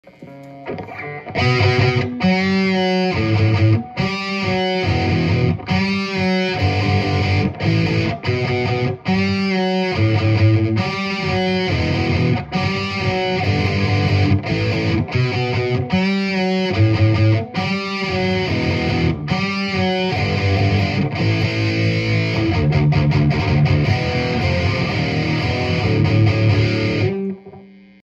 Bogner Uberschall Bedroom levels
Uber at bedroom levels.

Signal chain
Mayones Duvell Black Limba; Savage Drive; Uber Rev Blue; Uberkab; iPhone